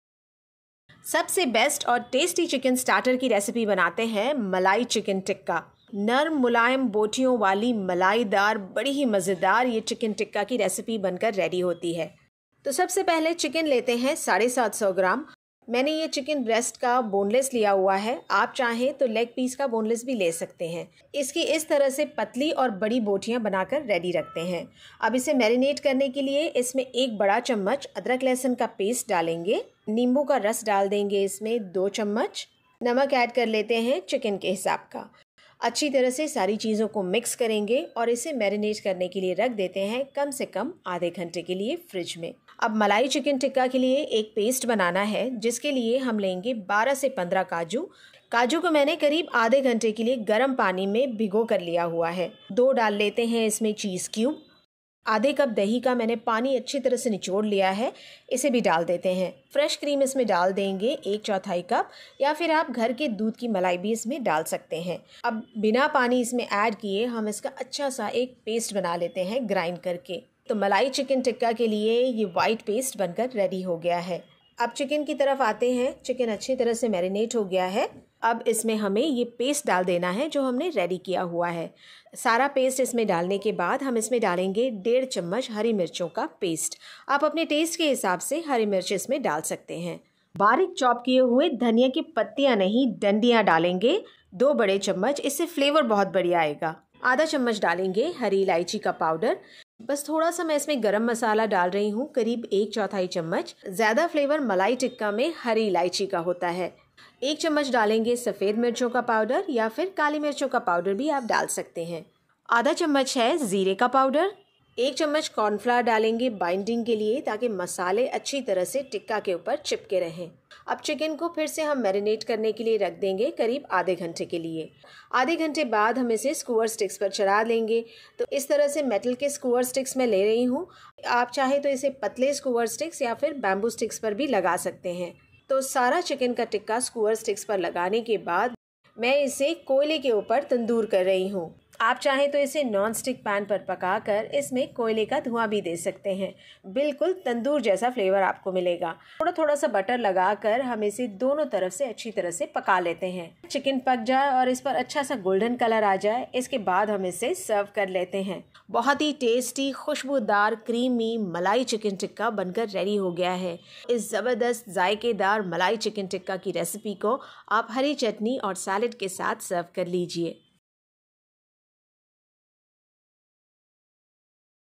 Soft And Juicy Chicken Tikka Recipe | Murg Malai Tikka Recipe
Ingredients:
Soaked Cashew Nuts - 12 to 15
Cheese Cubes - 2
Hung Yogurt - 1/2 cup
Fresh Cream - 1/4 cup
Green Chili Paste - 1.5 tsp
Chopped Coriander Stems - 2 tbsp